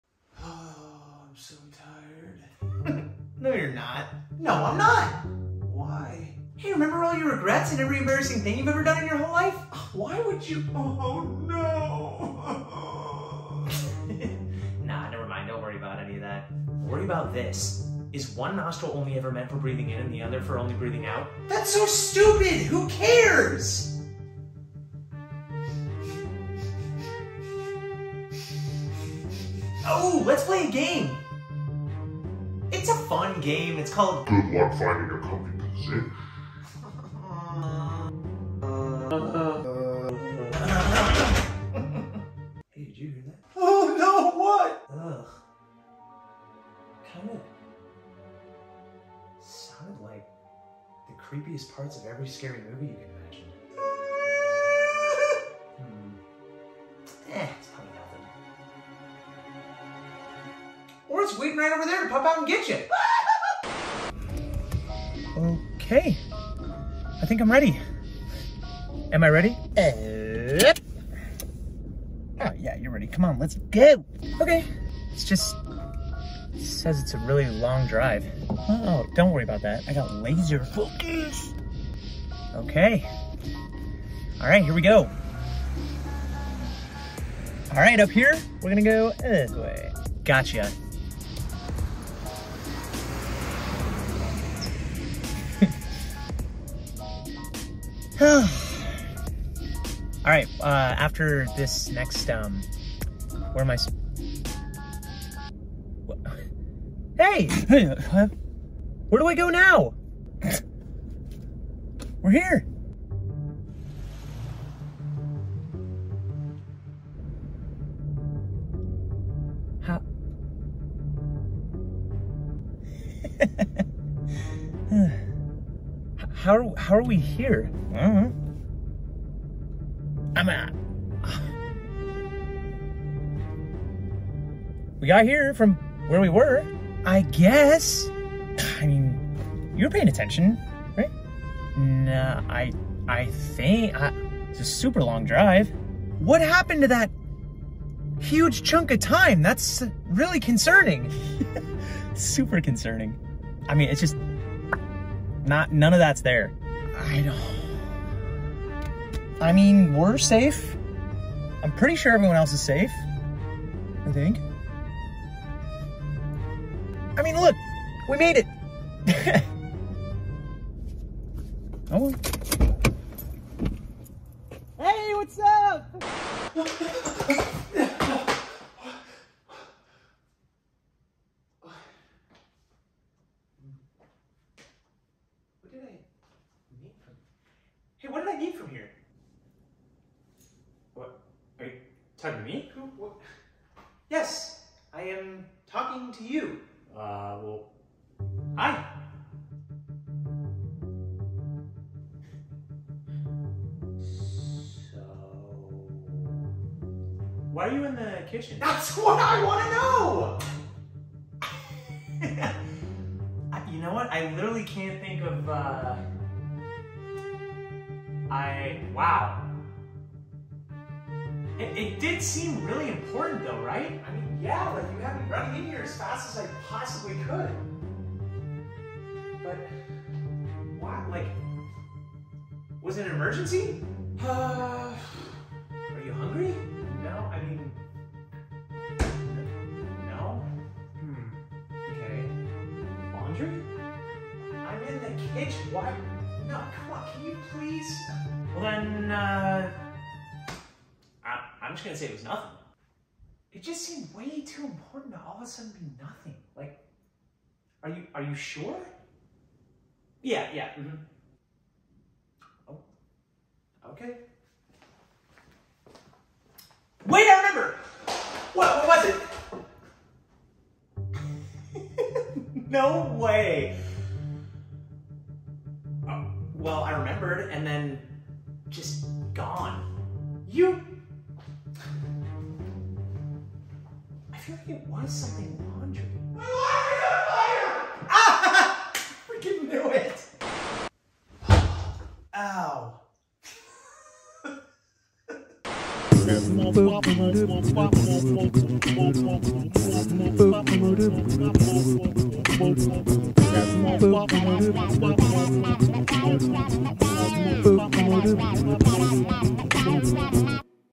When Your Brain Works Against You
This is my first longer skit series I've ever done on Youtube! Also I wrote/shot/acted in/edited it fully by myself! I really hope you enjoy it! It would mean a lot if you like it and tell me if you want to see more in the comments below!!
APPRECIATE YOU!!
Follow my friends!!
If you made it this far, comment: bleep bloop blorp!!